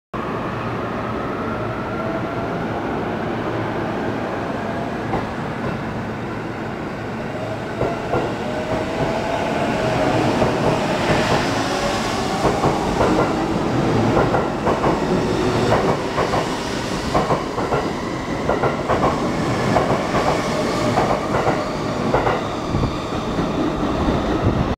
2014年11月2日 JR東日本常磐線E233系
我孫子行き松戸駅発車